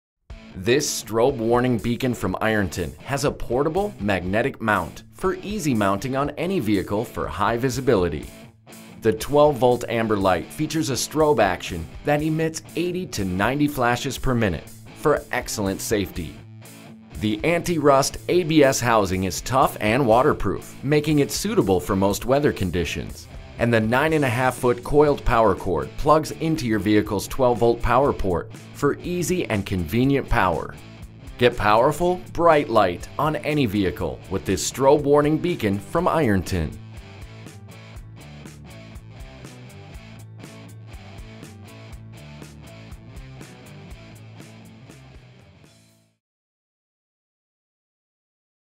Ironton Magentic Mount Strobe Warning Beacon - 12 Volt DC, Amber
The 12V DC Ironton Magentic Mount Strobe Warning Beacon features a powerful magnetic base for easy mounting on any vehicle. Strobe action emits 80-90 flashes per minute. Amber lens.